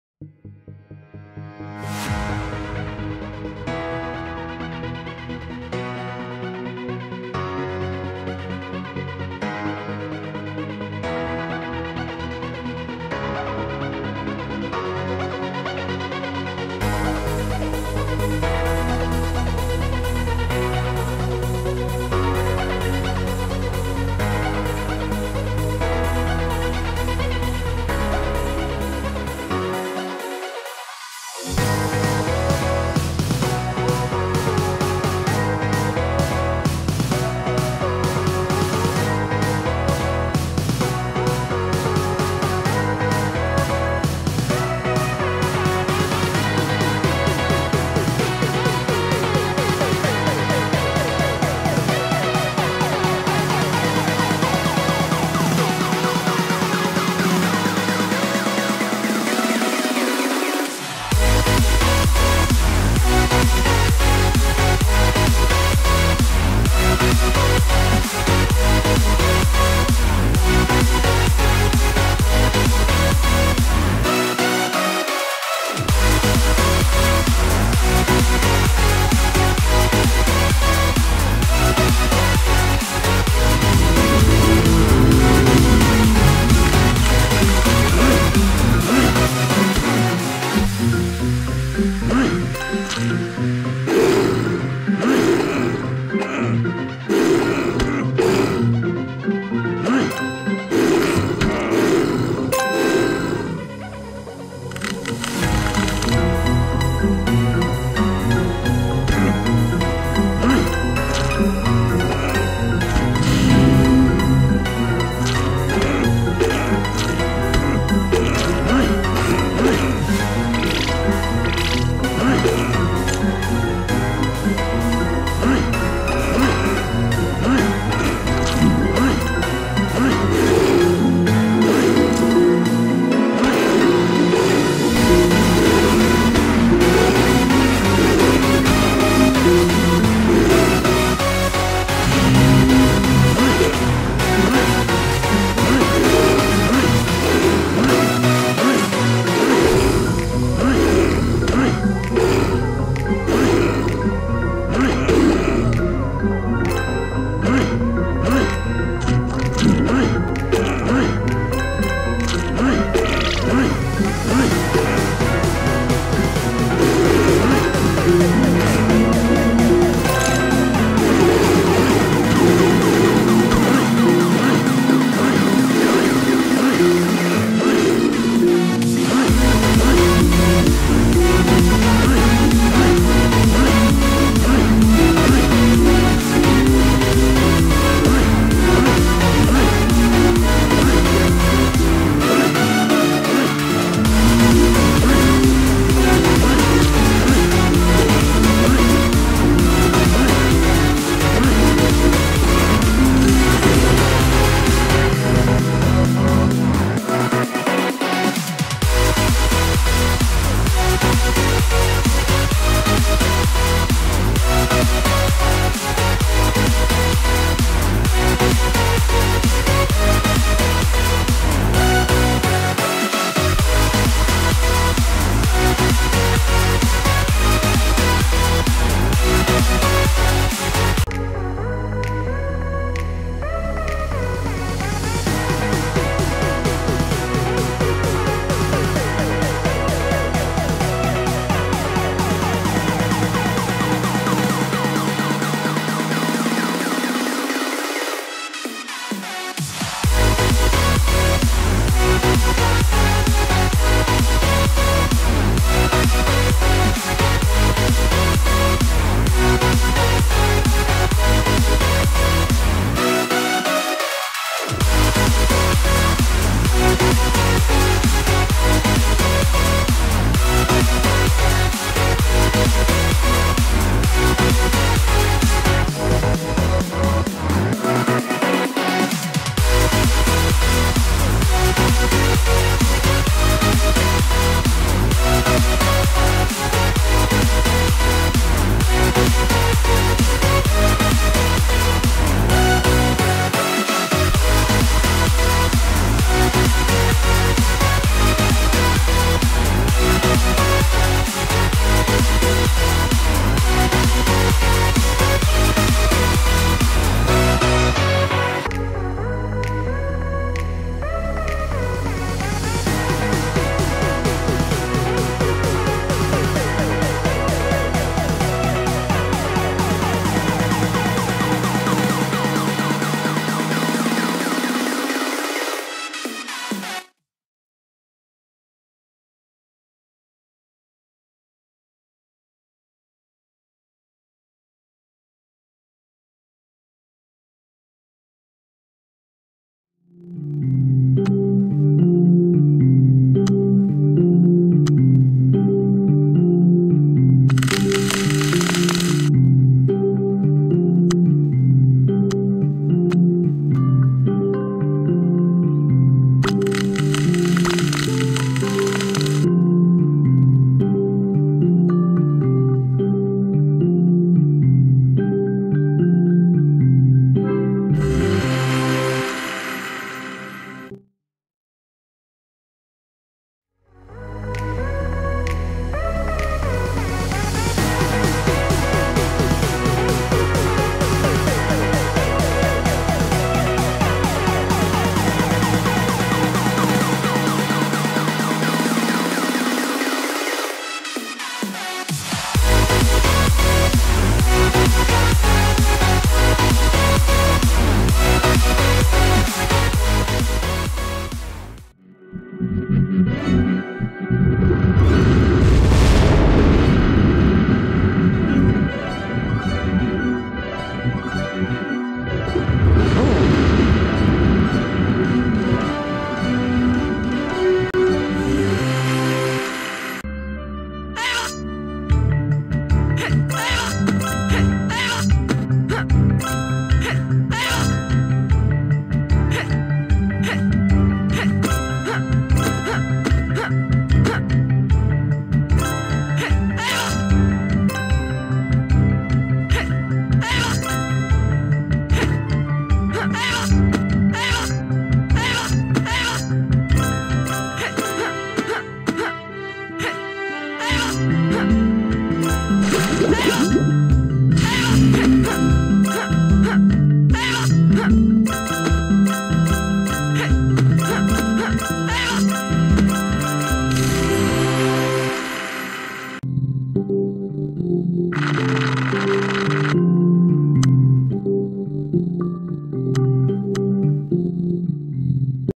The Sandbox: Evolution for steam has been released!
If you enjoy the mobile game The Sandbox evolution, then you will enjoy the steam version even more! Here is the link to the game (link) Anyway, I hope you gained some information, and you enjoyed it! Thank you for viewing :D